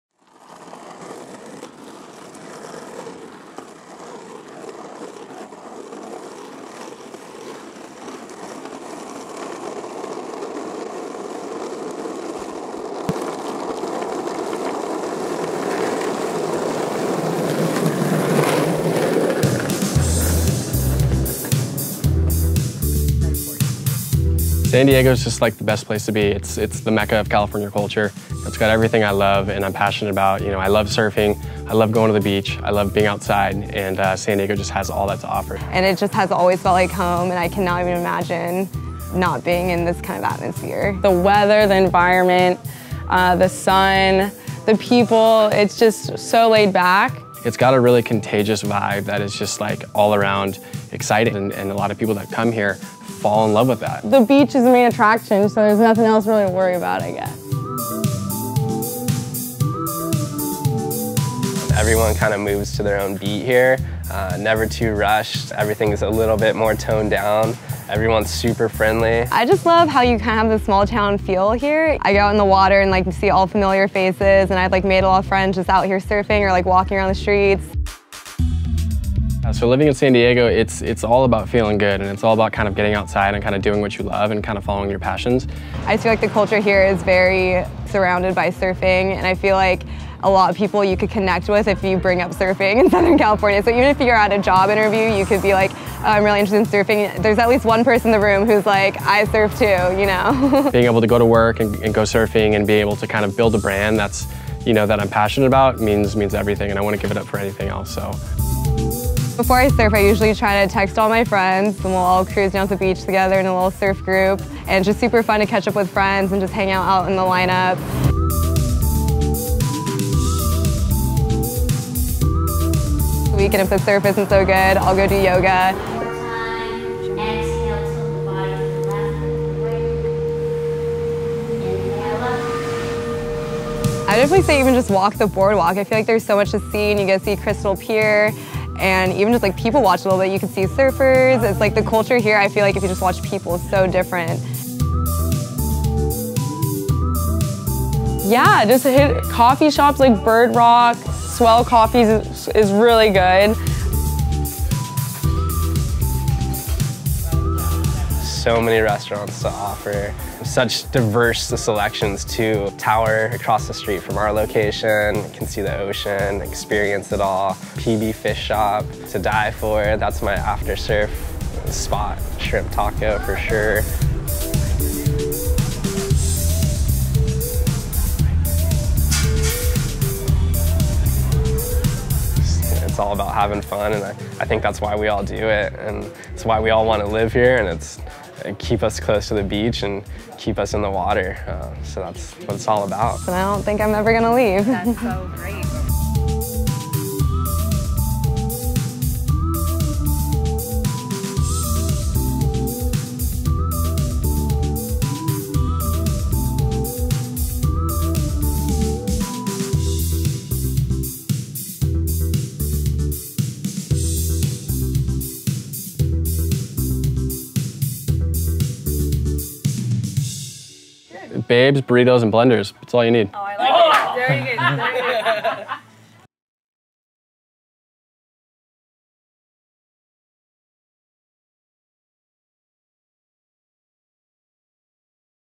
Visit San Diego: Pacific Beach
Considering a trip to Pacific Beach in San Diego? Let some locals tell you why they love it. Check out this video to learn what makes life in Pacific Beach so unique!

For more info on learning to surf and surf lessons in San Diego - check out San Diego Surf School